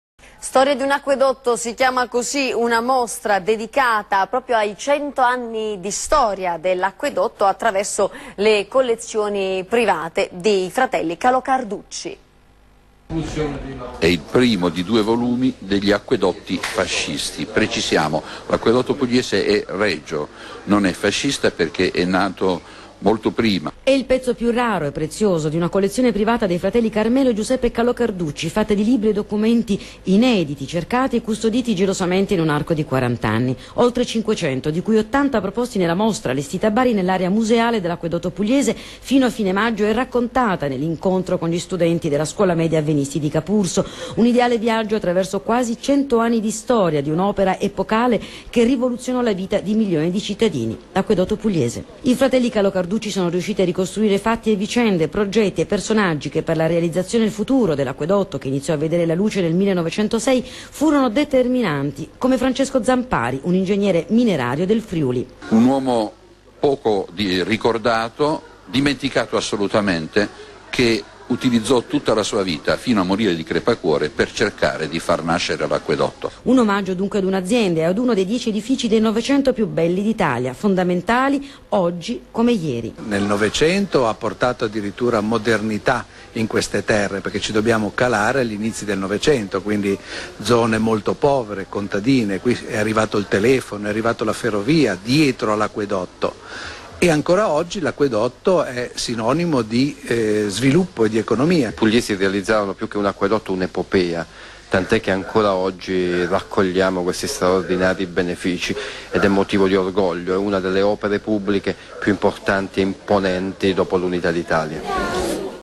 Storie di un acquedotto
Una mostra presentata a Bari attraverso le collezioni private dei fratelli Calò Carducci.
Data: 18/04/2012  - Edizione:   16:45
Rete: TGNORBA24

"Storie di un Acquedotto": l'Acquedotto Pugliese in un viaggio del tempo attraverso le collezioni private dei fratelli Calò Carducci

Dal 21 aprile al 27 maggio 2012, nello storico palazzo dell'Acquedotto Pugliese, a Bari, in Via Cognetti, 36, è possibile visitare, tutti i sabati e le domeniche, dalle ore 10.00 alle ore 13.00, la mostra "Storie di un Acquedotto".

La mostra propone attraverso due collezioni private, "frammenti di storie" relativi all'Acquedotto Pugliese. Un'opera, epocale, destinata a cambiare la storia civile di milioni di cittadini.

Il percorso della esposizione si riferisce ad un arco di tempo di circa cento anni: dalla seconda metà del diciannovesimo secolo fino agli anni cinquanta del novecento.

La mostra propone ottanta immagini e documenti storici che raccontano una storia di sacrifici ma anche un'epopea esaltante.

Come per incanto ritornano fatti e vicende, speranze e progetti dal 1867 al 1902 e poi personaggi determinanti per la storia dell'Acquedotto e della Puglia come Camillo Rosalba, Francesco Zampari, Matteo Renato Imbriani, Gioacchino Poli, Ercole Antico, Gaetano Salvemini, Nicola Balenzano, Gaetano Postiglione.

Oltre alla mostra, durante la visita si possono ammirare gli splendidi arredi e decori realizzati dall'artista Duilio Cambellotti, un'interessante esposizione di strumenti antichi, immagini descrittive delle "opere maestose" di Aqp ed una corografia con il tracciato degli schemi idrici, in modo da poterne cogliere l'estensione sul territorio.